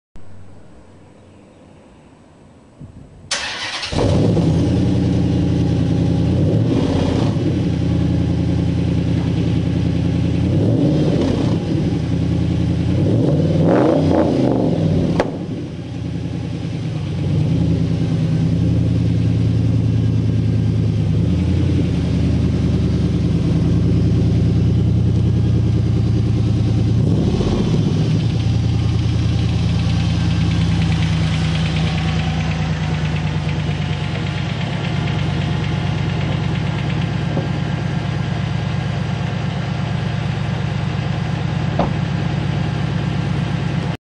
Dodge Challenger SRT8 MBRP Exhaust Cat back
Startup , rev, and back up of the Challenger.